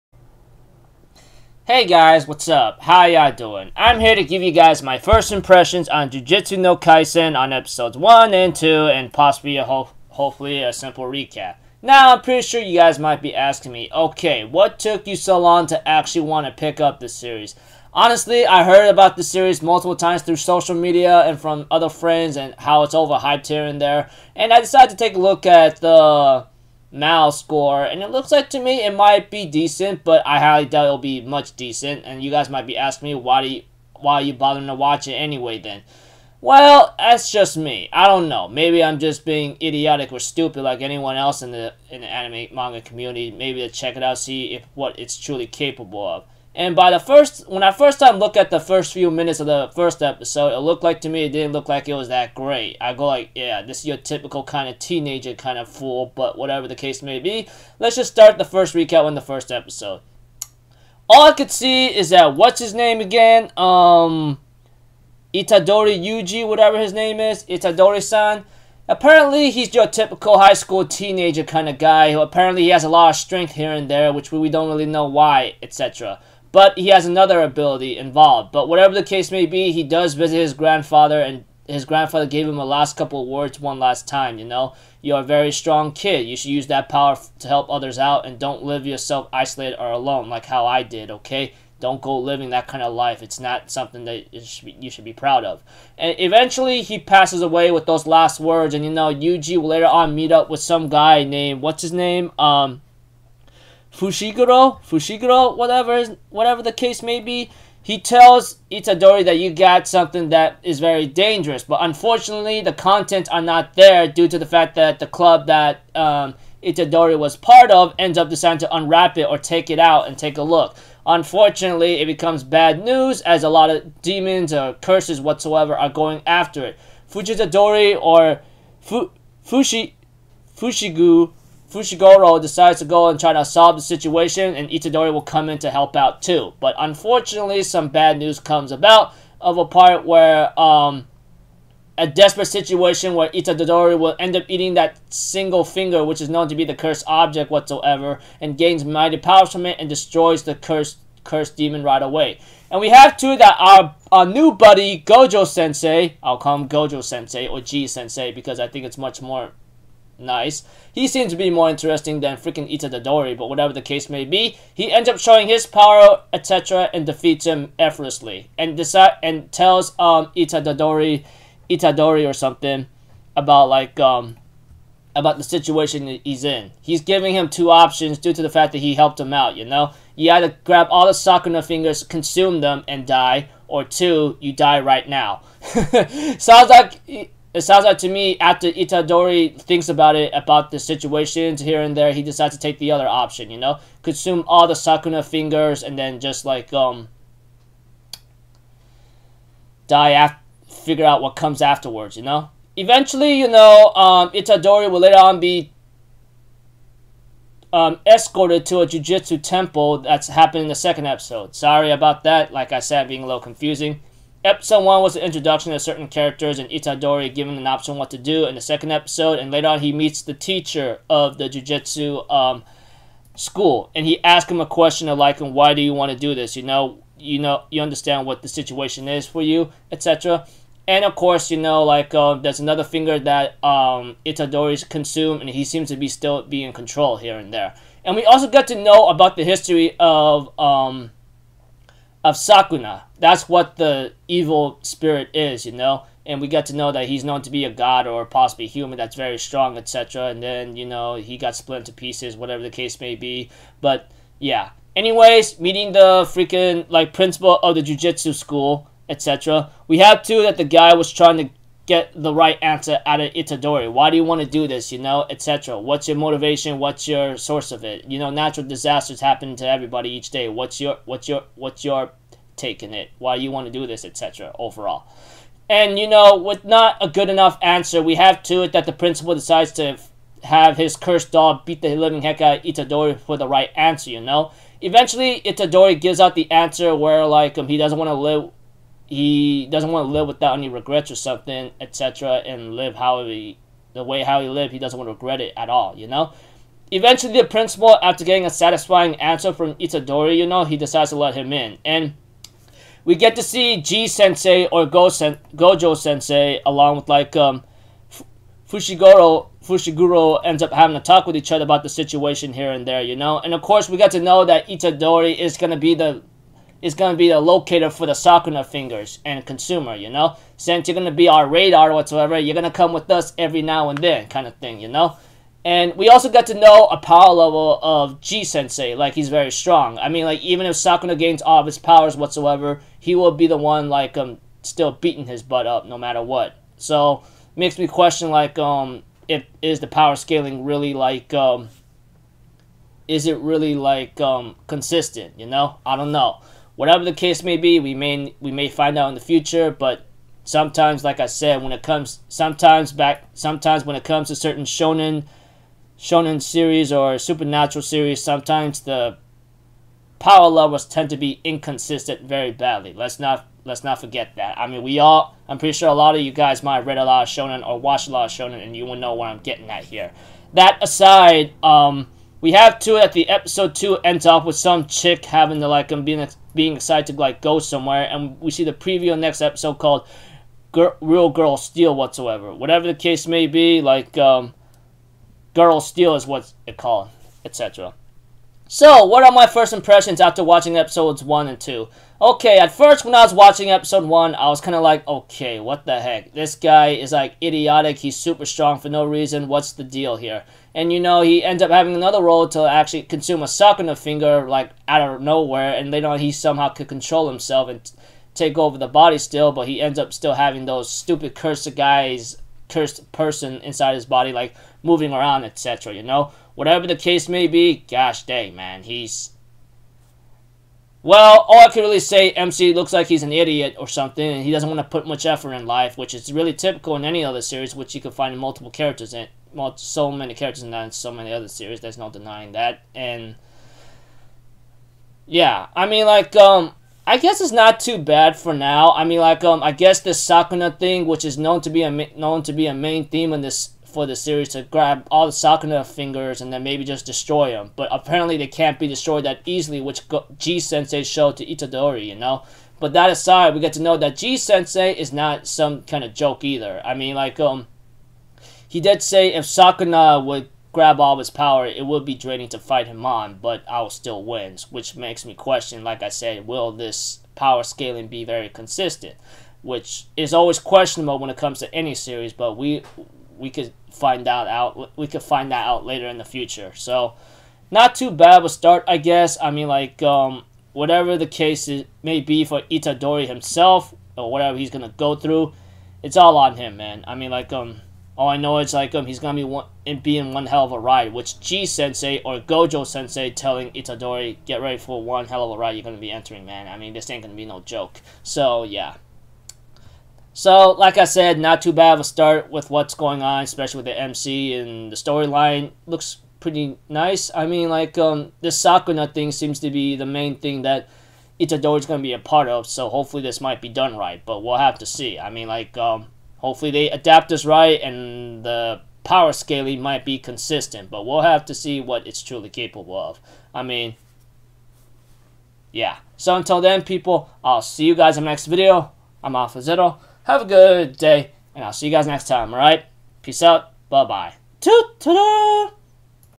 Jujutsu Kaisen 呪術廻戦 Episodes 1 and 2 First Impressions!
Took me time to think on it, but I decided to watch the first two episodes. Anyways. Hopefully this does not go bad down the road.